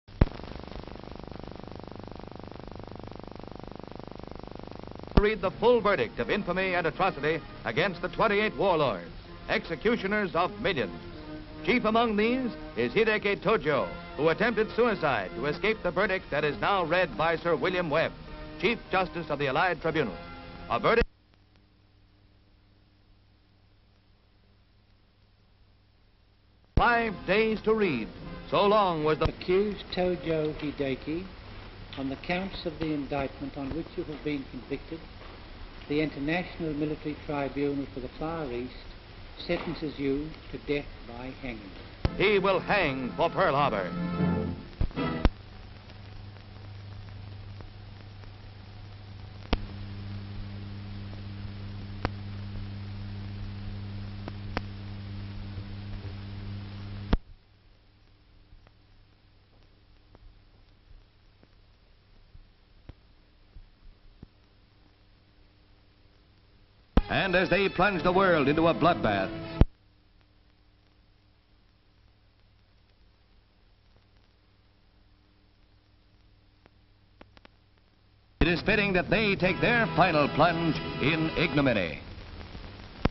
Les criminels de guerre japonais condamnés à mort par pendaison
Description : Six criminels de guerre japonais dont Hideki Tōjō sont condamnés à mort par le Tribunal militaire international pour l'extrême-Orient.
Les prisonniers dans le boxe des accusés. Le président du Tribunal, William Webb, prononce la sentence. Scènes de pendaison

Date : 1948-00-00